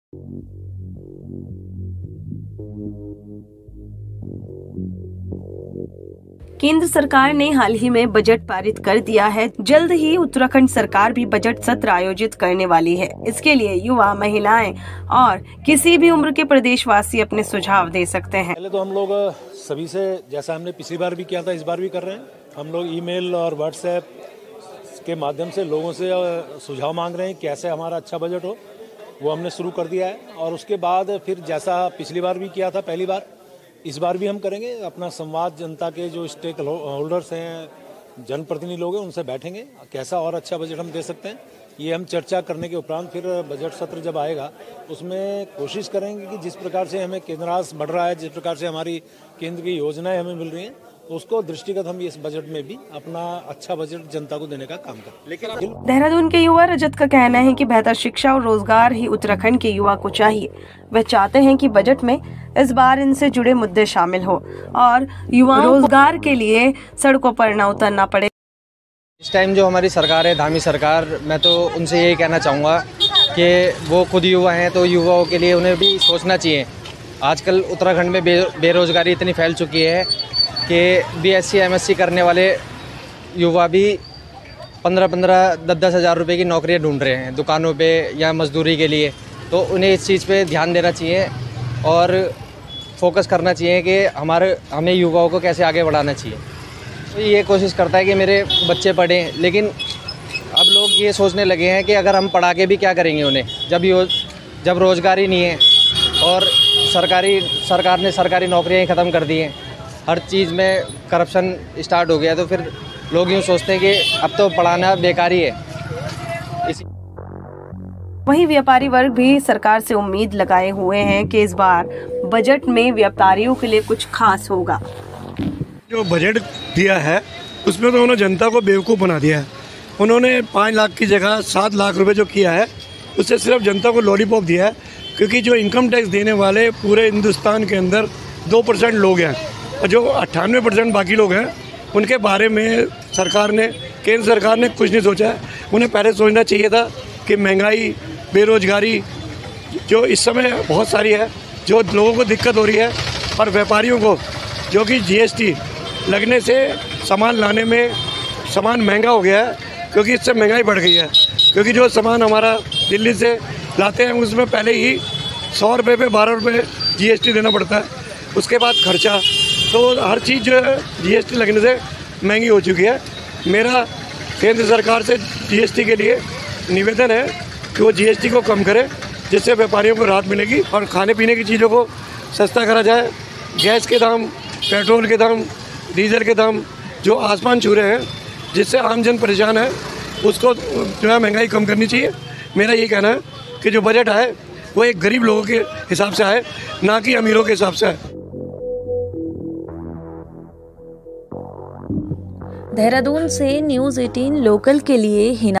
Uttarakhand Budget: आपको उत्तराखंड बजट में क्या चाहिए? सरकार तक ऐसे पहुंचाएं अपनी बात, पर जल्दी
राज्य का कोई भी नागरिक 27 फरवरी तक अपना सुझाव ऑनलाइन या ऑफलाइन माध्यम से दे सकता है.